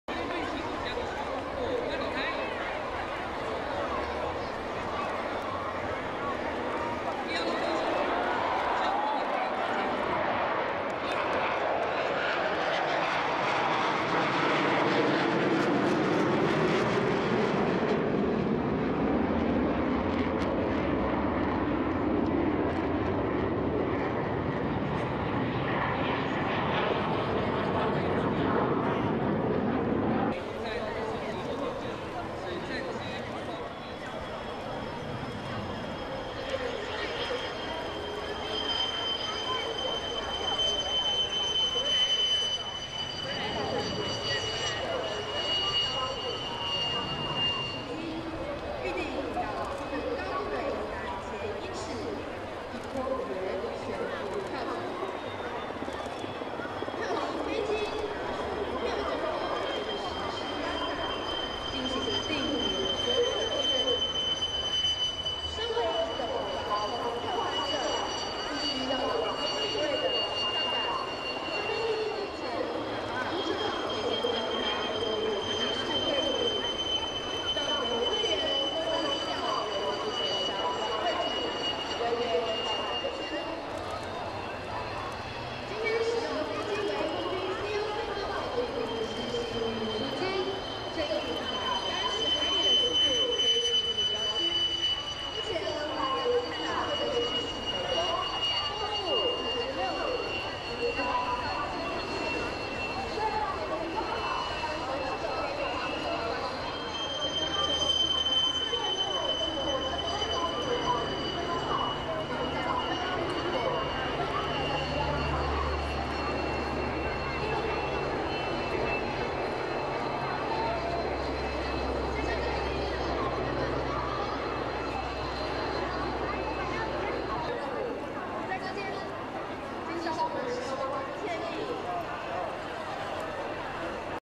花蓮基地 營區開放活動 F-16 五機空中分列式衝場落地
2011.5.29 中華民國空軍401聯隊 花蓮基地 全民國防知性之旅 營區開放活動 F-16 五機空中分列式衝場落地 ROCAF Hualien Air Base 2011 Fighter Aircraft  Show F-16 Air Show and Landing  - 版權所有 請勿使用於商業用途.